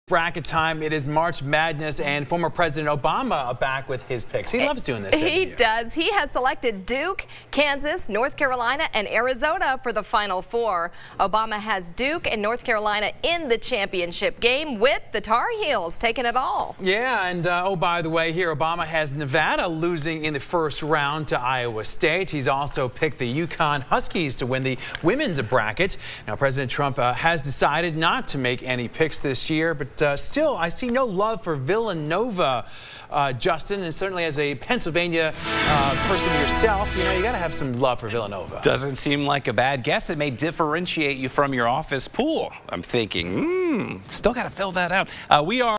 Former President Obama releases March Madness bracket
Former President Barack Obama has released his March Madness bracket for 2017.